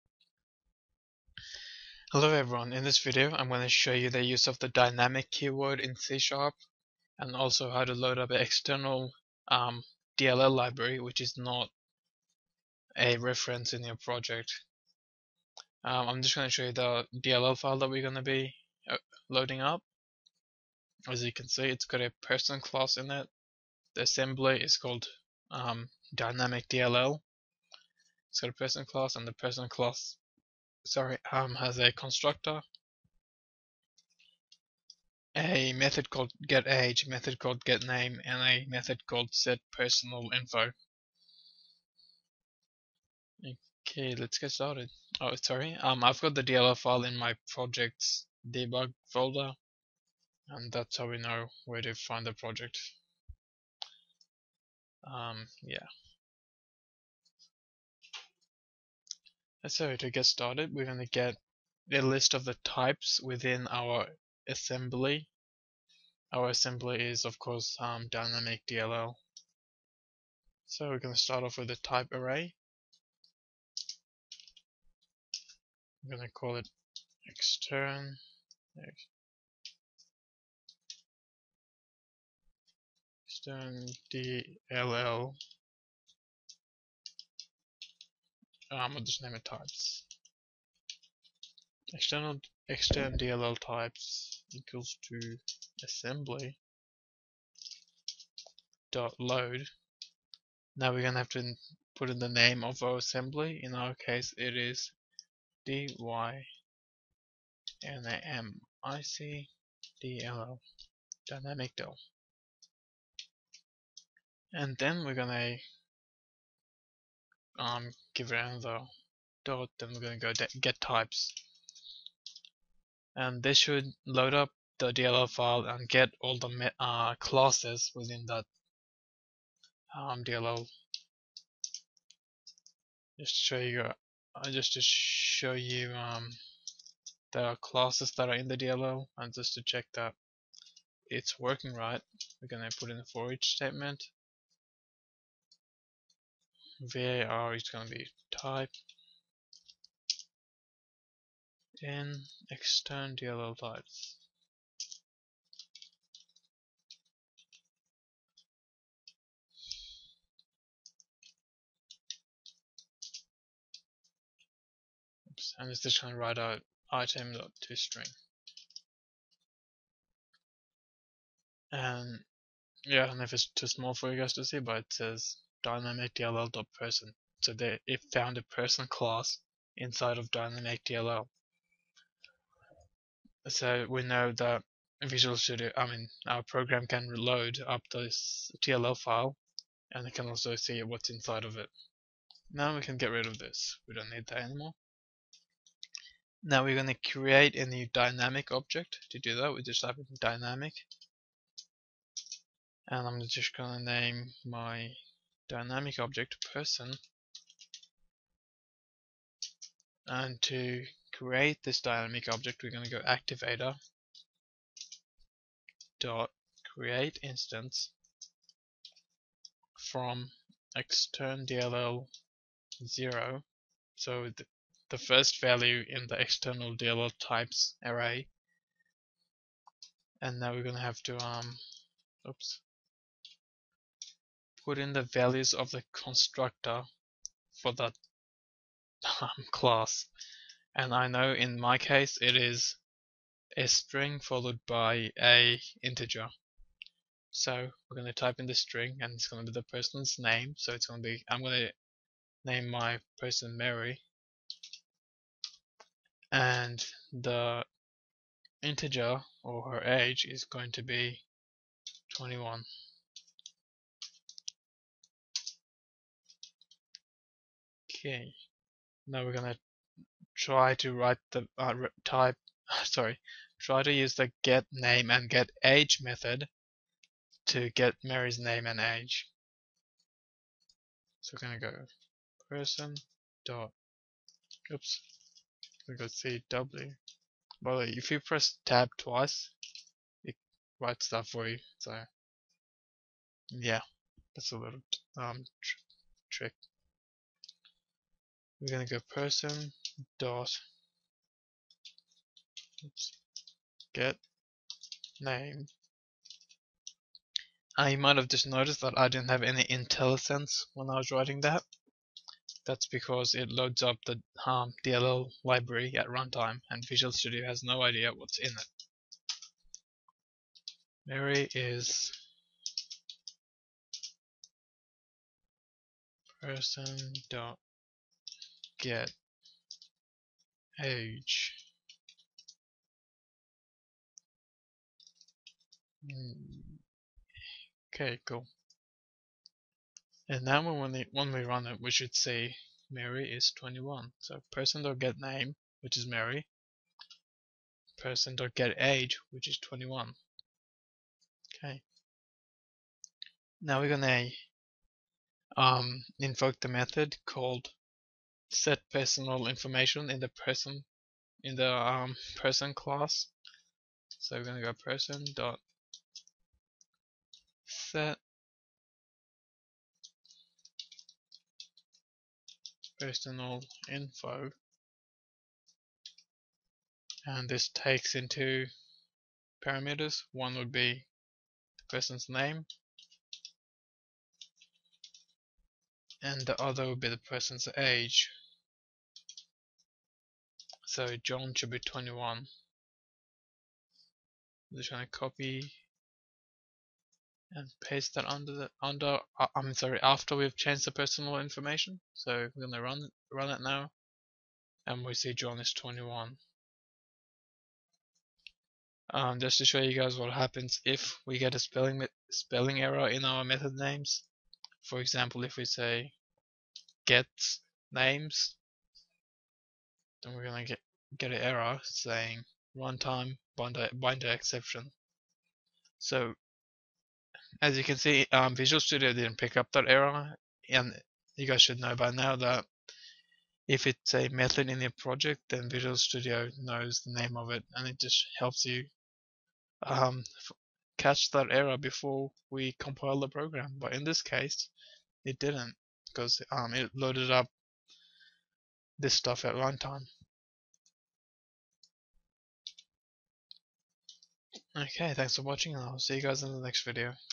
C# dynamic and loading DLLs at runtime
How to use the dynamic keyword and how to load a DLL file at runtime.